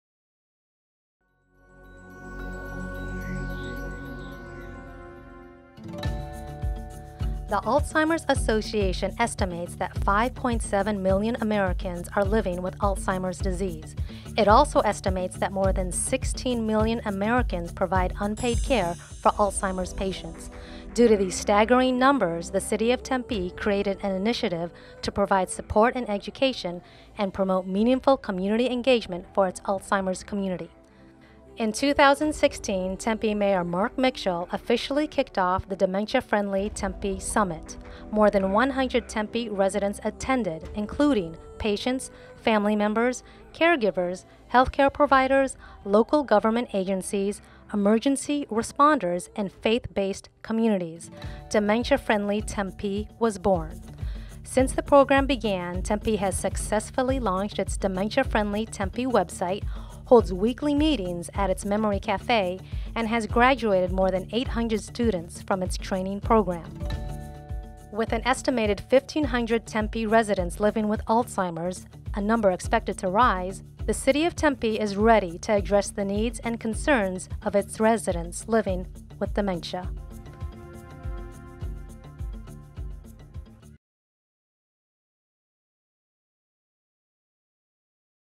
2018 Desert Peaks Public-Private Partnership Award Winner: Dementia Friendly Tempe
This award is presented to the group or project that has demonstrated a commitment to regionalism through a public-private partnership.